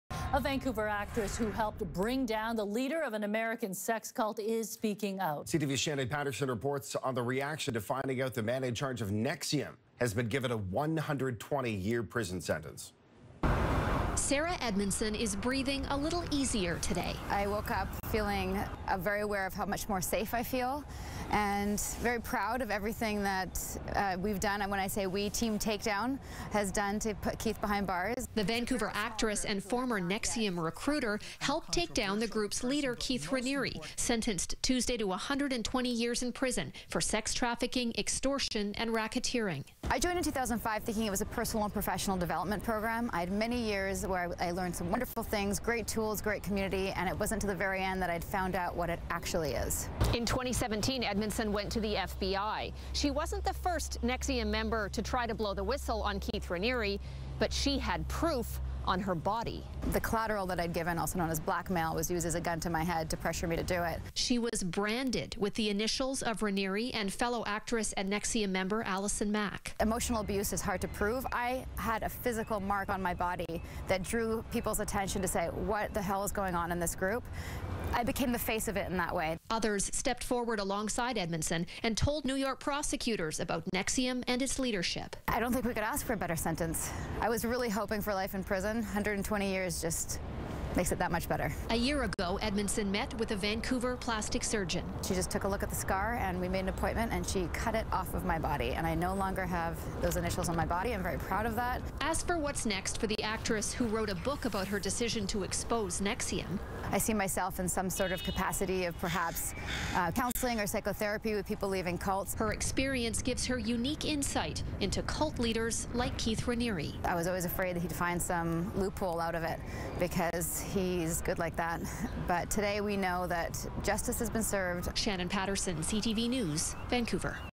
Local victim of NXIVM cult reacts to leader's sentence
A Vancouver actress who helped bring down the leader of an American sex cult is speaking out in reaction to his sentence.